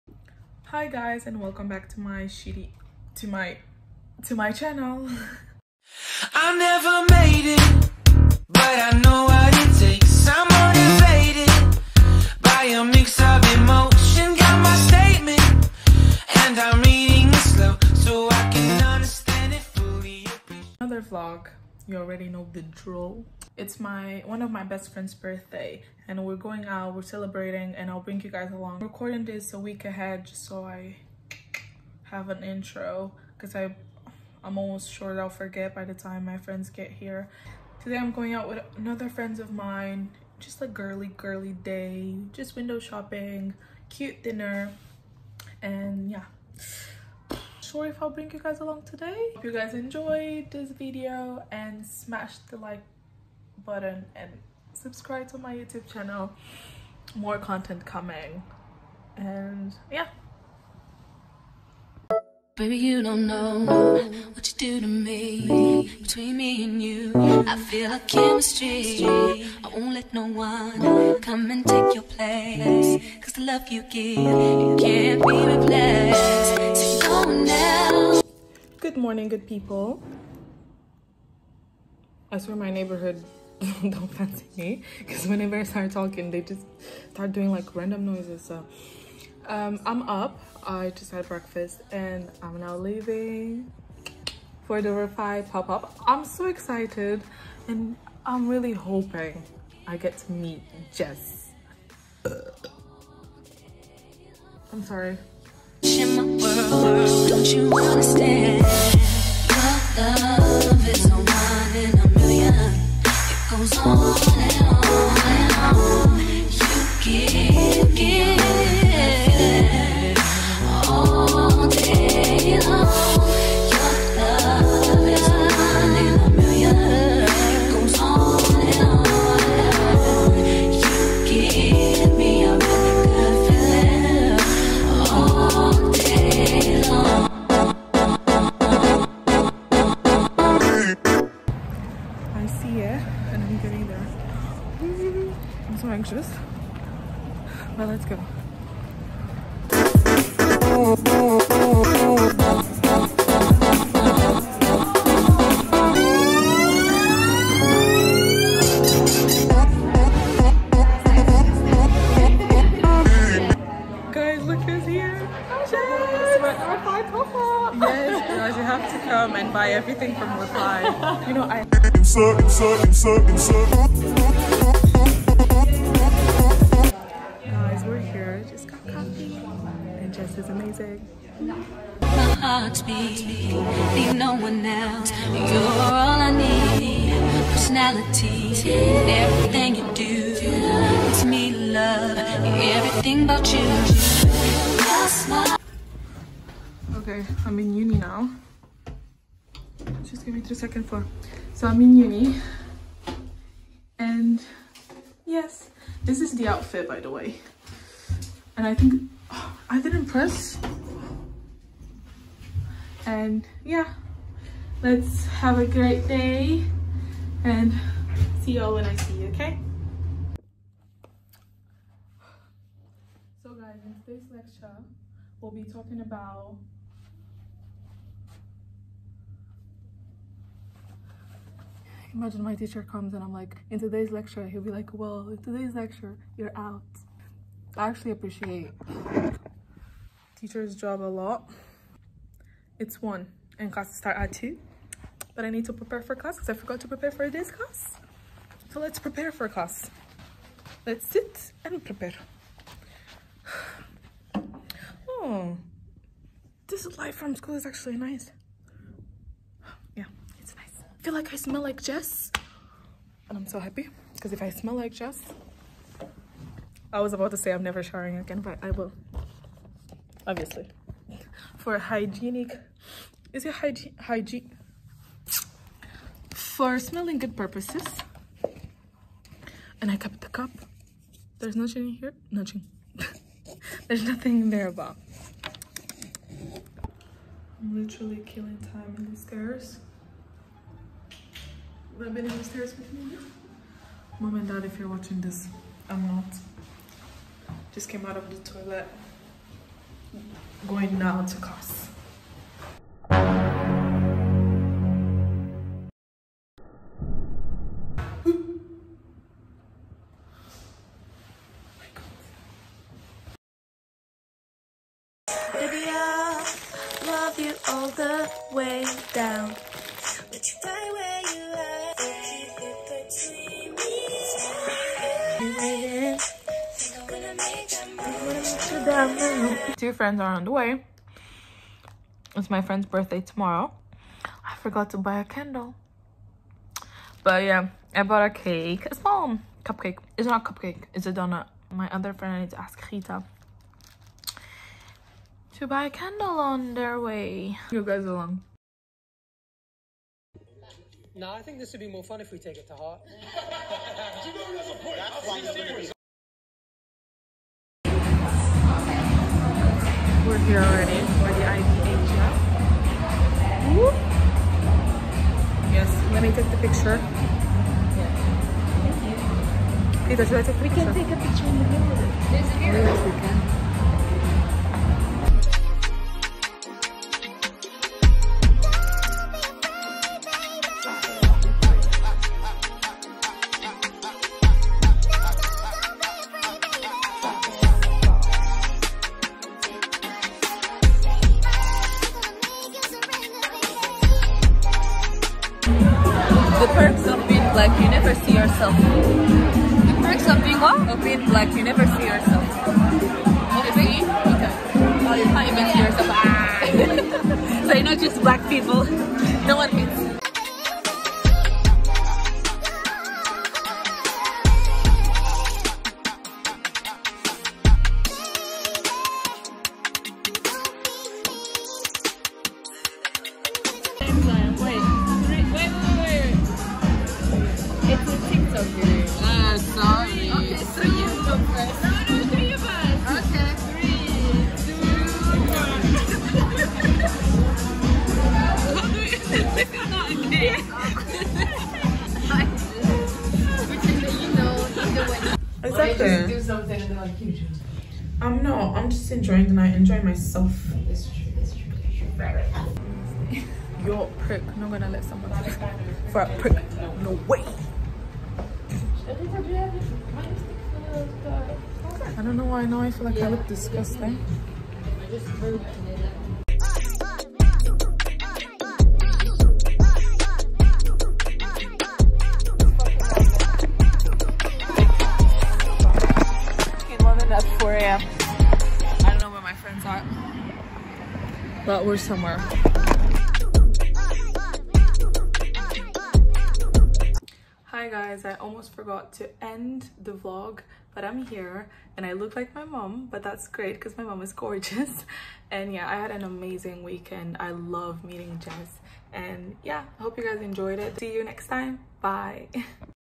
I MET JESS HUNT | REFY POP UP STORE | NIGHT OUT IN LDN
Hey you,

I met JESS HUNT (still screaming inside) she’s a marvelous woman and her team is so dreamy. It was also the birthday of one of my closest friends and I kindly took you guys along. Hope you enjoy 💞